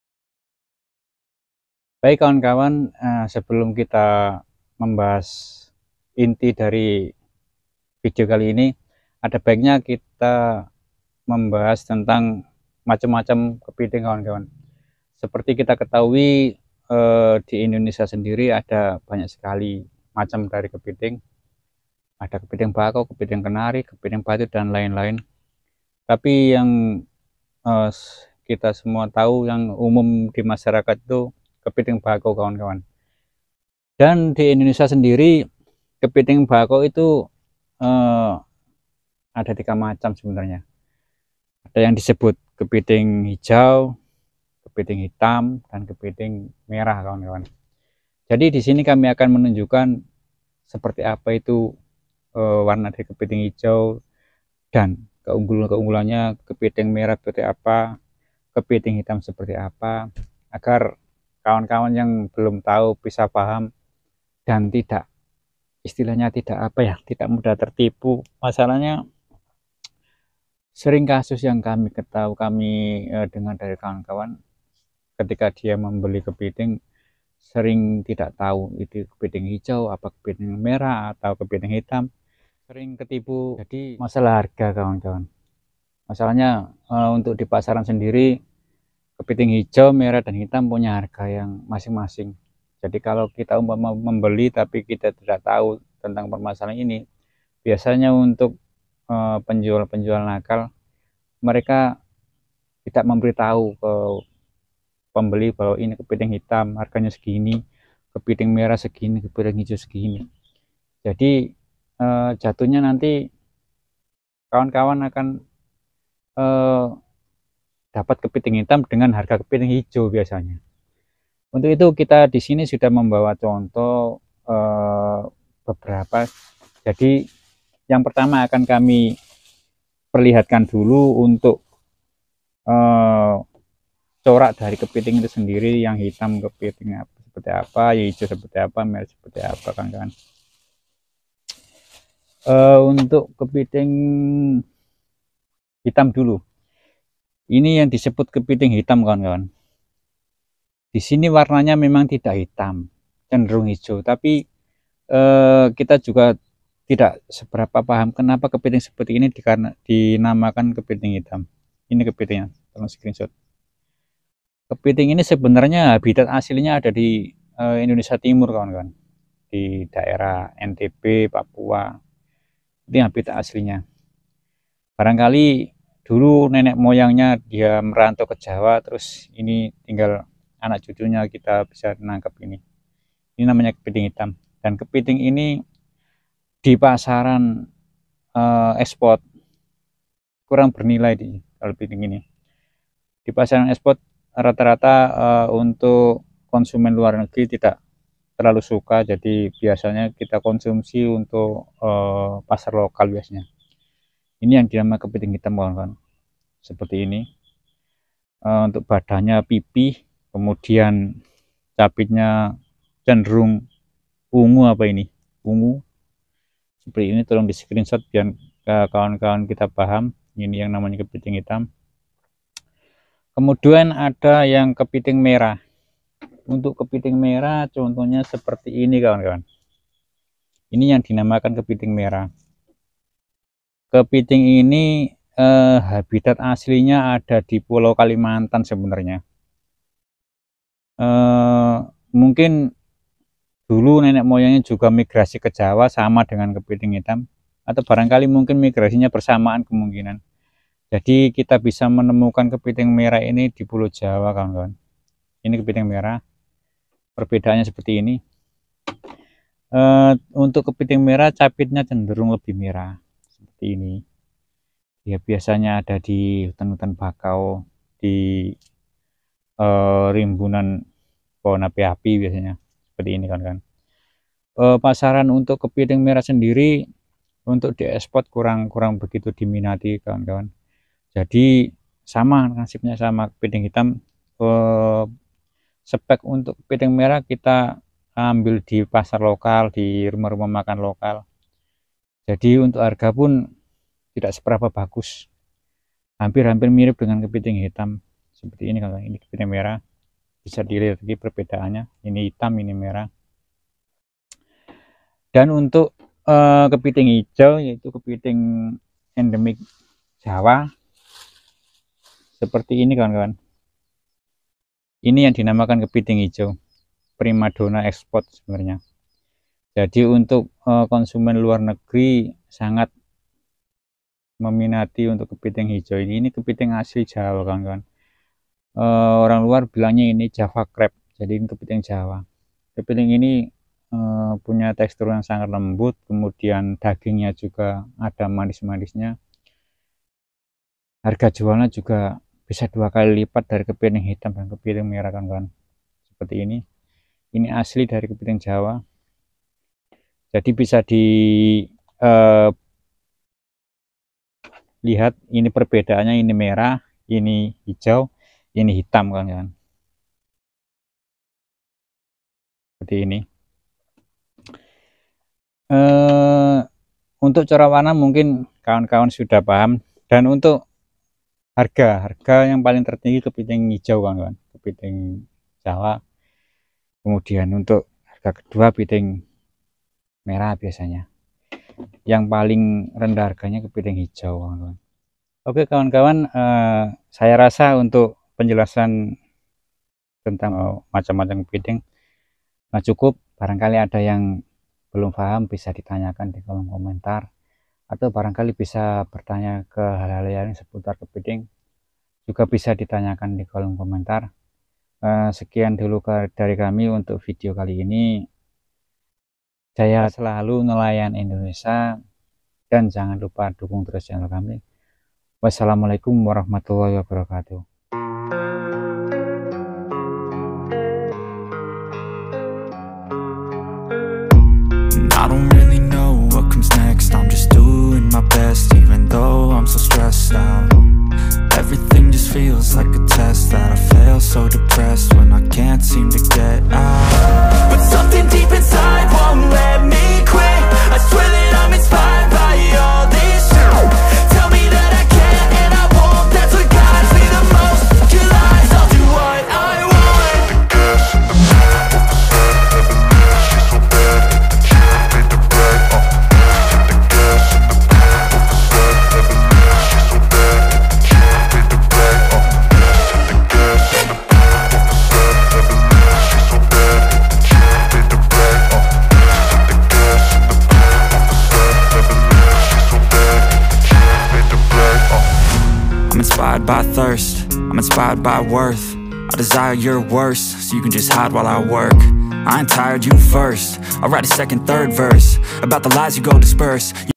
kepiting Jawa vs kepiting papua
perbedaan kepiting Jawa vs kepiting papua.
kepiting Jawa banyak macamnya kepiting bakau kepiting merah kepiting hijau